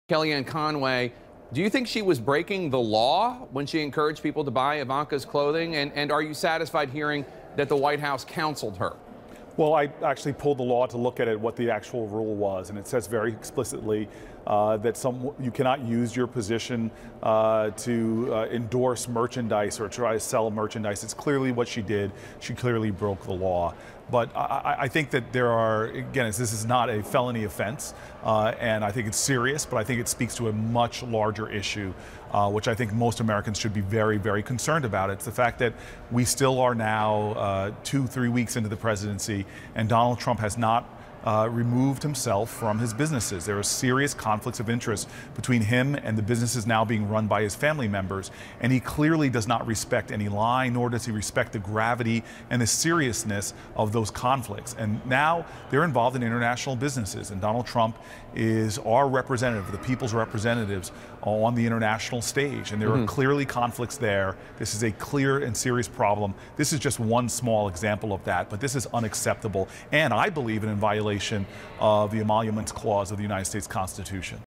Booker: Kellyanne Conway clearly broke the law
Sen. Cory Booker says that senior adviser Kellyanne Conway broke the law during a televised interview when she urged viewers to purchase Ivanka Trump-branded products.